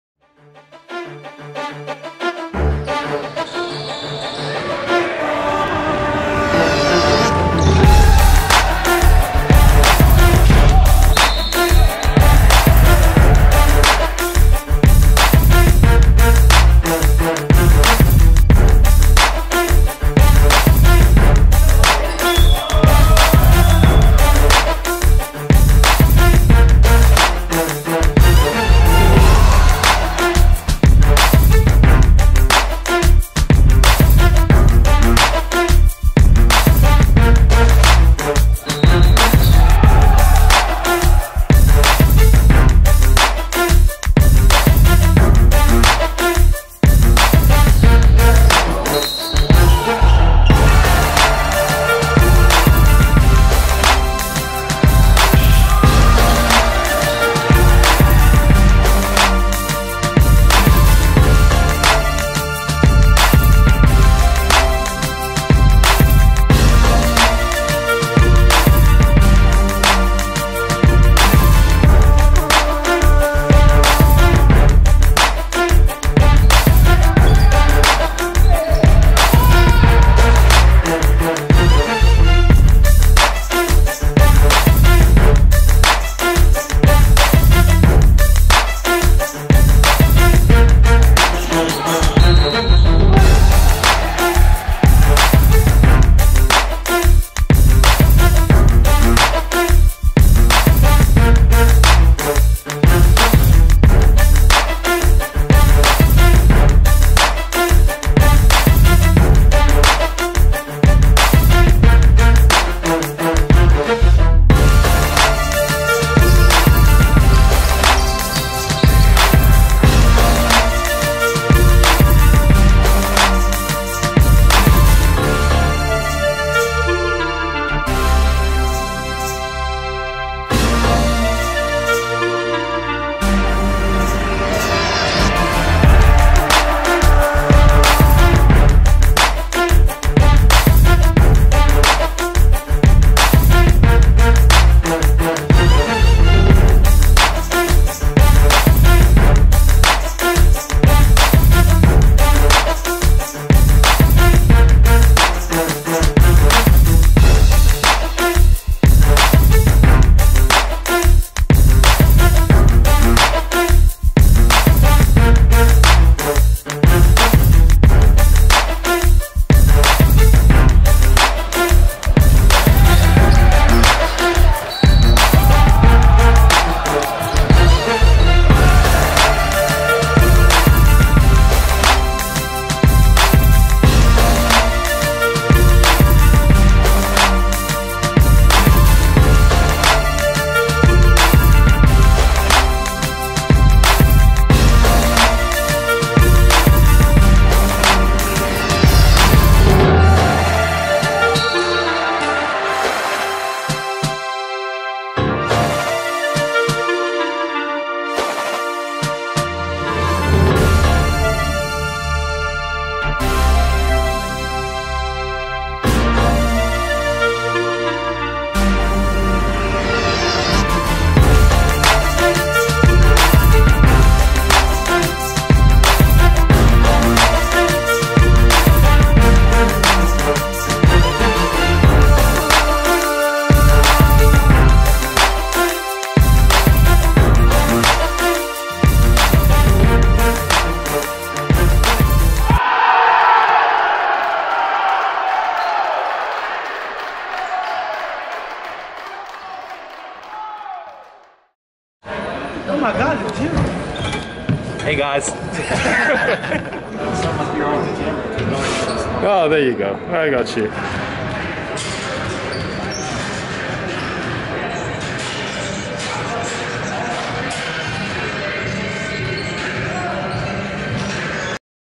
Cornell College JV Mixtape!
Highlights of Cornell College Men's Basketball Junior Varsity team in the 2021/2022 season.

Thumbnail: Wallace Schmotzer

Roster:
Jacob King
James Stimson
Trey Totheroh
Cade Nelson
Charlie Robinson
Tanner Ingle
Austin Kelly
Mason Meyer
Brextyn Beard
Giannis Psaroudakis
Ray Borchert
Wallace Schmotzer